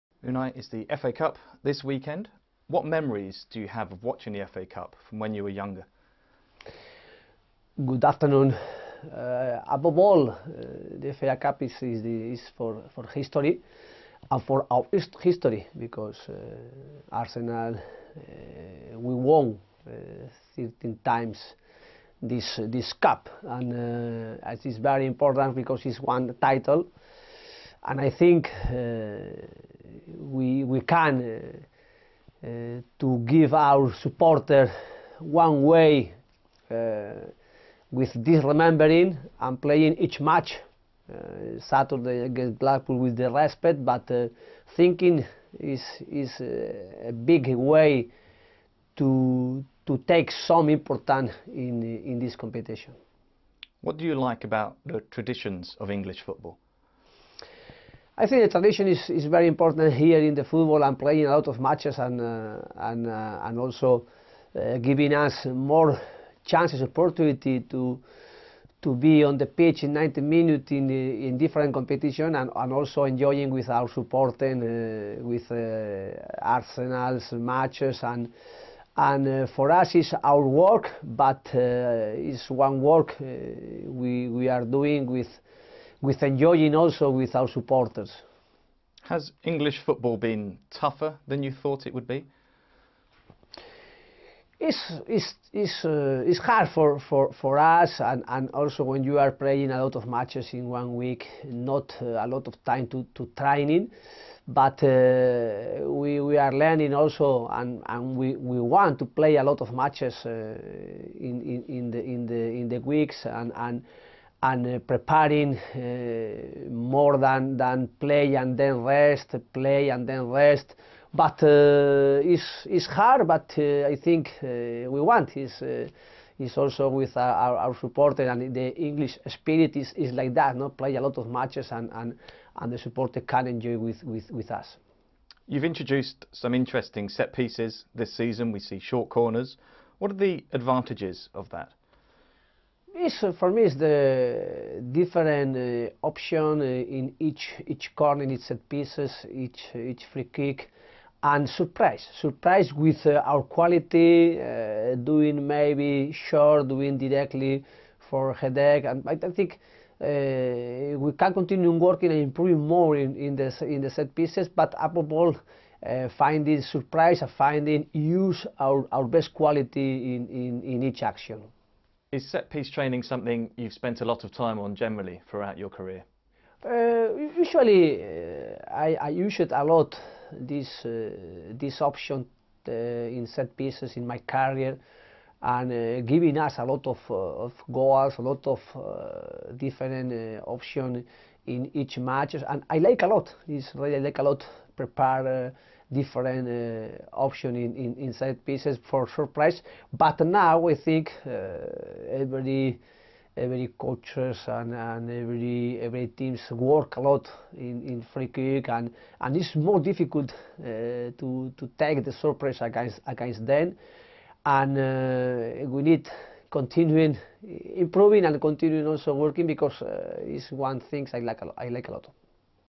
Emery: The FA Cup is an important part of our history
Unai Emery speaks to Arsenal Player ahead of our FA Cup clash with Blackpool on Saturday